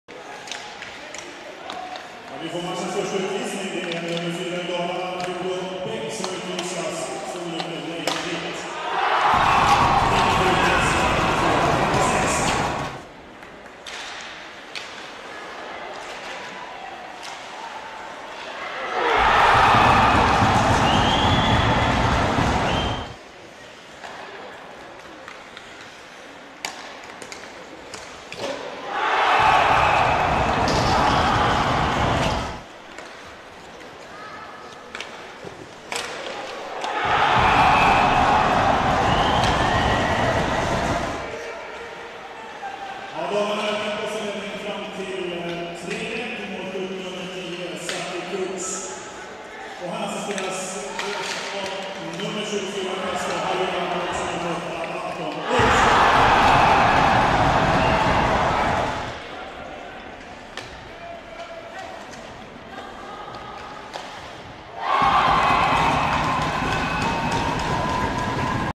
TV-Pucken: Dalarna - Värmland 4-2
Målen från matchen Dalarna - Värmland.
(1-1, 3-1)
Skottstatistik: 30-23
Matcherna sänds på SvenskHockey.TV